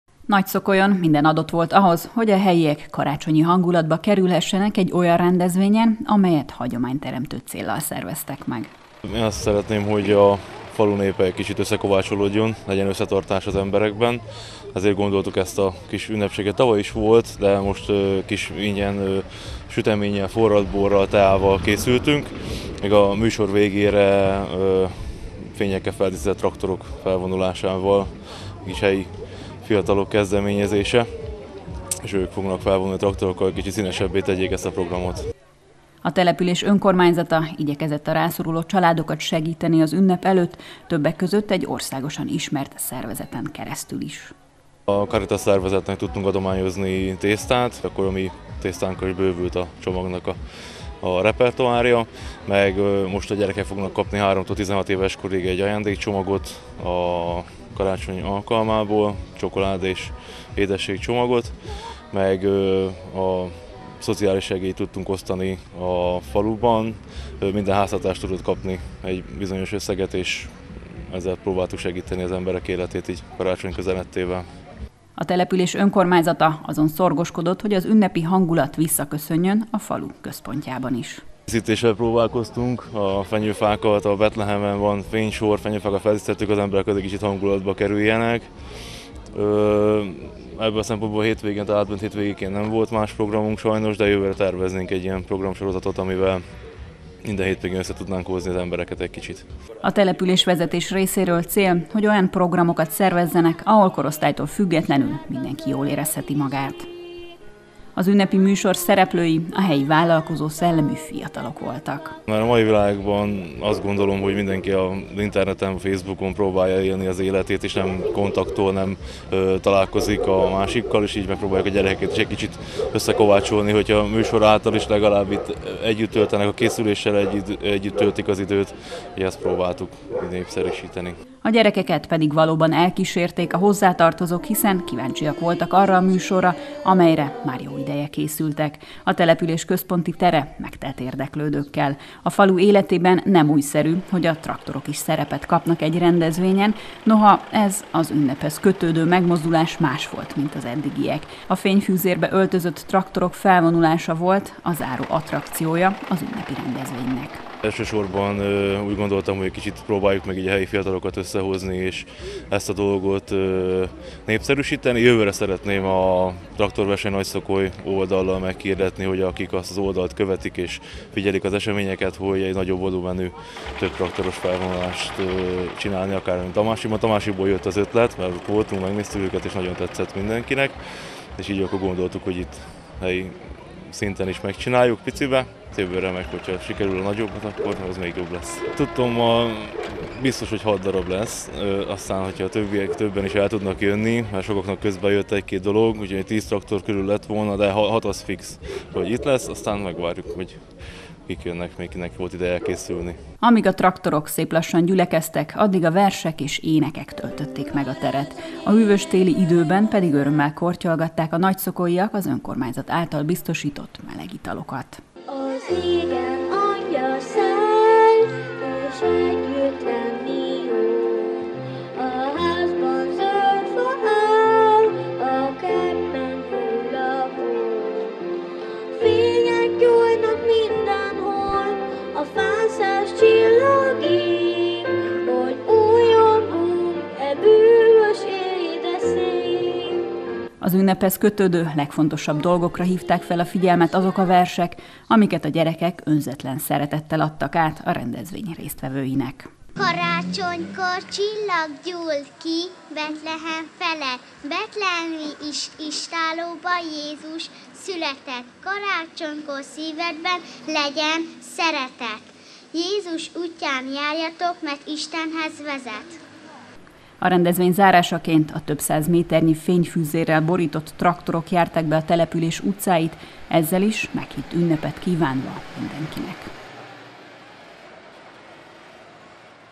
Fényfüzérrel díszített traktorok és szeretetvendégség - Nagyszokoly, falukarácsony
A közös ünneplés Nagyszokolyon sem maradt el. Karácsonyi dallamoktól volt a hangos a település központja, ahol a gyerekek igyekeztek igazi ünnepi hangulatot teremteni műsorukkal. A szeretetvendégségről az önkormányzat gondoskodott, ahol a forró italok mellett felajánlásokból pogácsa és sütemény is várt a jelenlévőkre. A délutánt pedig a fényfüzérekkel feldíszített traktorok felvonulása zárta.